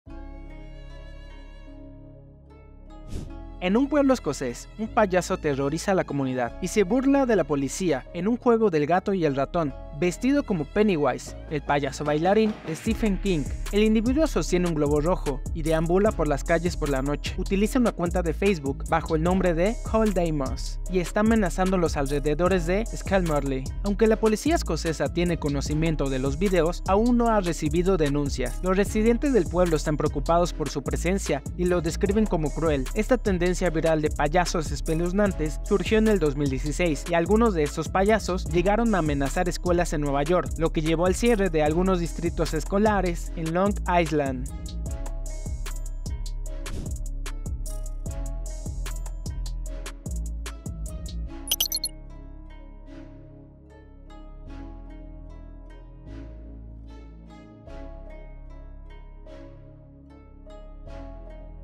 Misterioso payaso aterroriza a un pueblo escocés y reta a la policía a atraparlo
Un payaso vestido como "Pennywise de Stephen King, con pelo rojo, cuello y mallas con volantes, suele sujetar el infame globo rojo mientras desfila por la calle durante la noche.